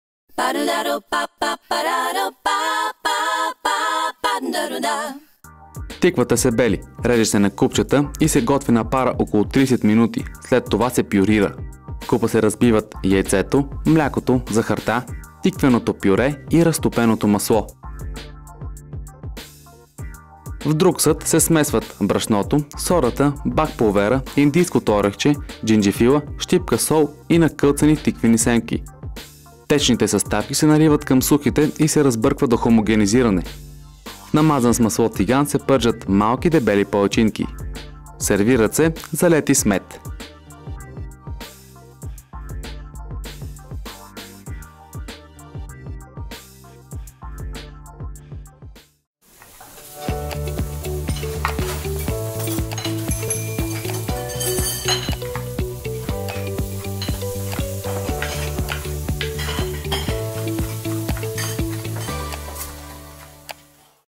Тиквени палачинки с мед | Бон Апети
Интересни и много ароматни палачинки с мед. Цялата рецептата може да видите на сайта на Бон Апети

Необходими продукти:
тиква „Цигулка“
прясно мляко
яйце
краве масло
брашно
захар
 бакпулвер
сода
сух джинджифил
индийско орехче
сол
белени тиквени семки
мед